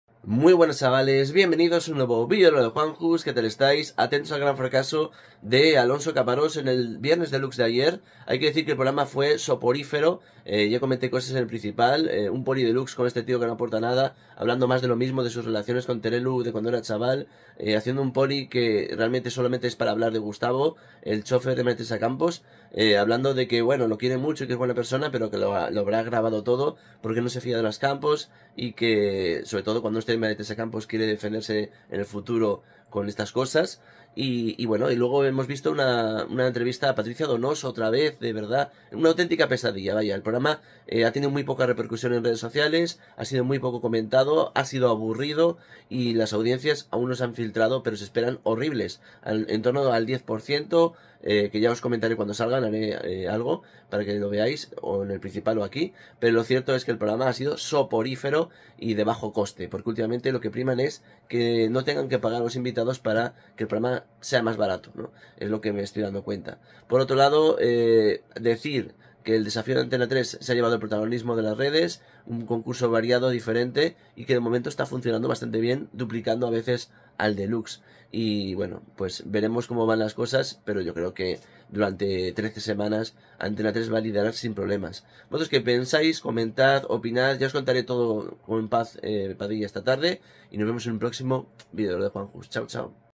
El gran fracaso de Alonso Caparrós en el Sálvame Deluxe / Viernes Deluxe de Jorge Javier Vázquez
El programa de telecinco con Jorge Javier Vázquez no ha sido el gran éxito que se esperaba en redes sociales ni en audiencias tampoco tras ver el resultado del mediafest de anteayer .
María Patiño , Kiko Hernández o Lydia Lozano estuvieron en el programa de Sálvame Deluxe antes llamado Sábado Deluxe .